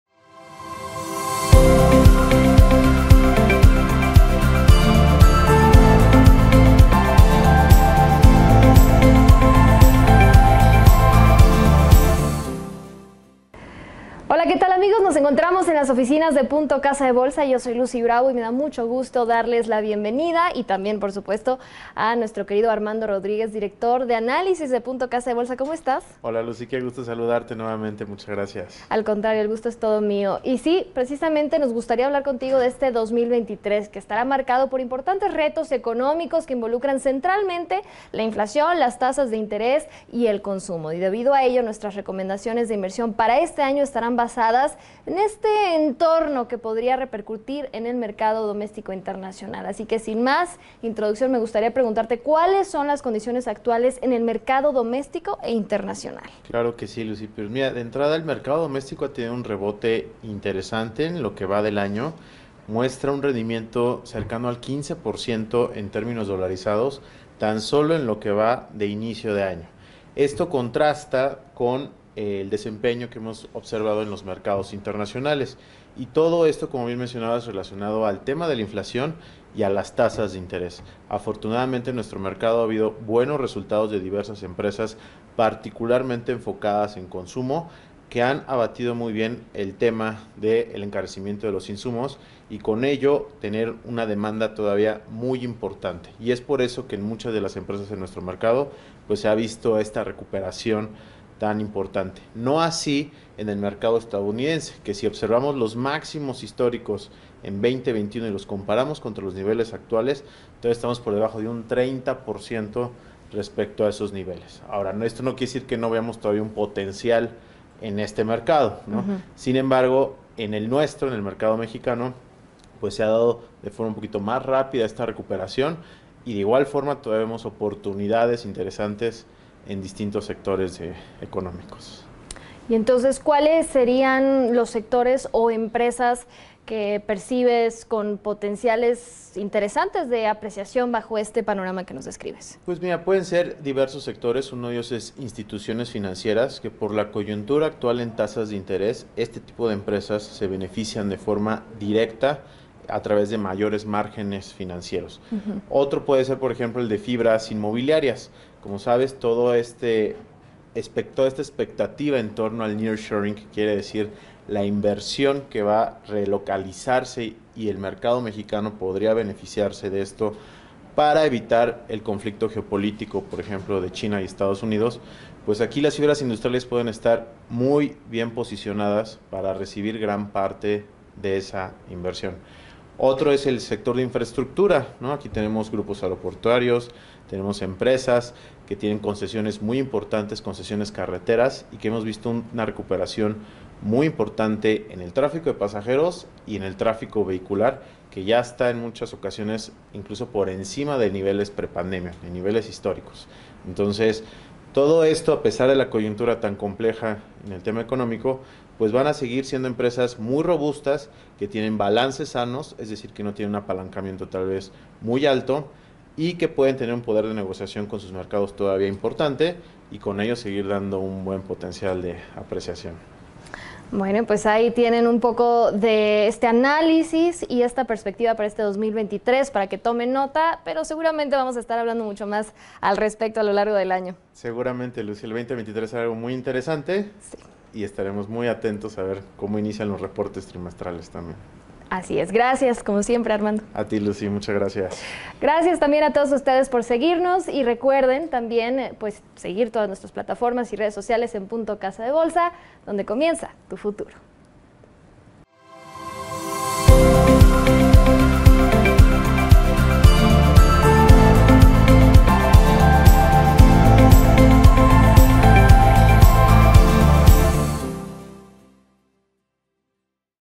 ¿Cuáles son las condiciones actuales en el mercado doméstico e internacional 2023?
Lucy Bravo se encuentra en las oficinas centrales de Punto Casa de Bolsa y en esta ocasión le realizó una detallada entrevista a Armando Rodríguez, Director de Análisis.

🟣 Platicaron acerca de los retos de las tasas de interés, el consumo y la inflación, así como los  sectores que vemos con más potencial.

Punto Casa de Bolsa 👌
Donde comienza tu futuro